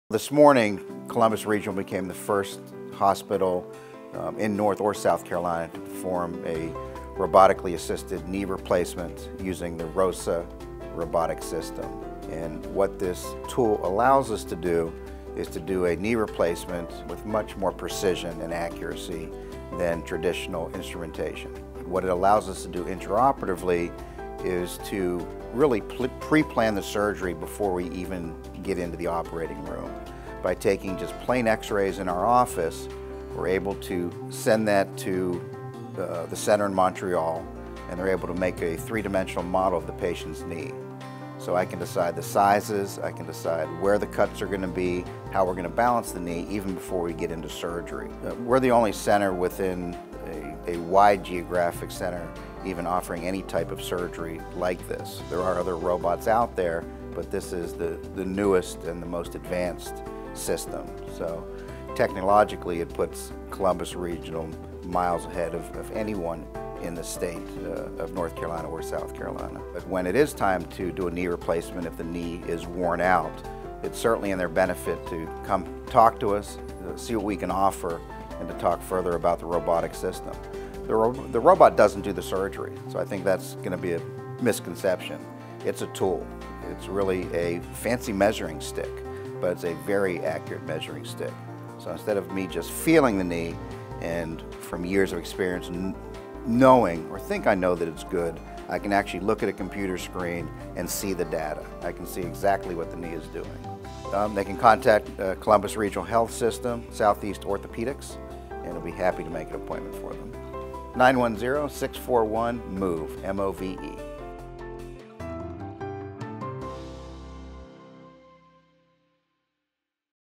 Robotically-Assisted Knee Replacement at Columbus Regional Healthcare System
Columbus Regional became the first hospital in North and South Carolina to perform a robotically-assisted knee replacement using the ROSA robotic system.